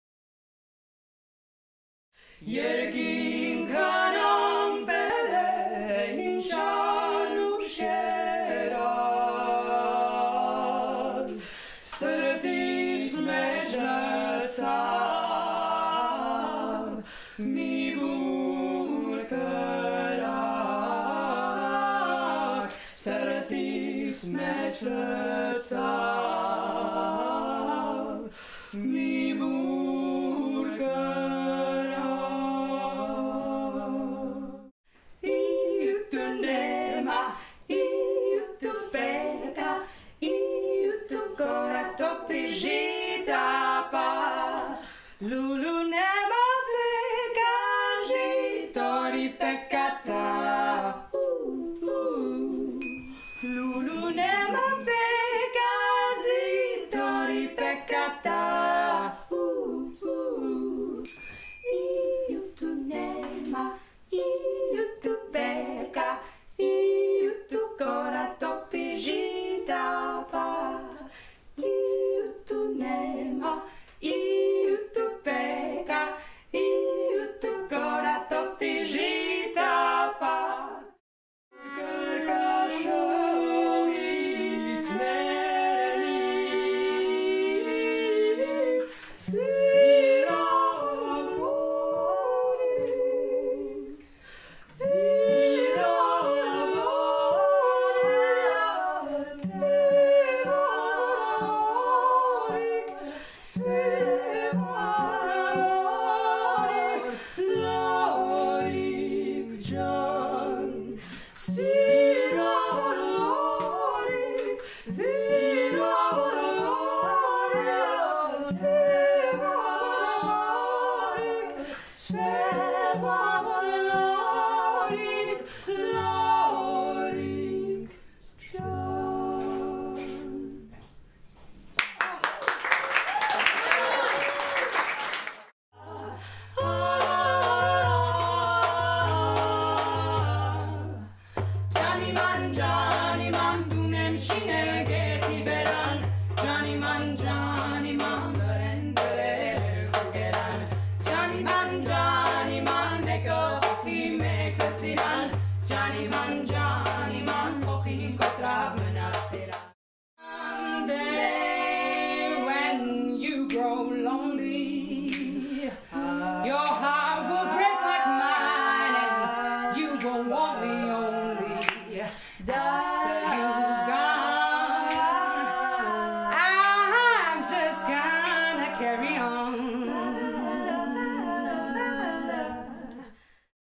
YY sisters
The vocal a cappella group YY sisters aims to preserve the traditional folk music of ancient  peoples by bringing new perspective and reverence to age old music. The scope of the group's selection is extremely extensive covering a large historical ground. The repertoire includes works by Armenian historian Movses Khorenatsi of the fifth century to English medieval carols and early American music. The group captures the listener with evocative, unique and modern arrangements while still being deeply rooted in oral traditional and early folk singing.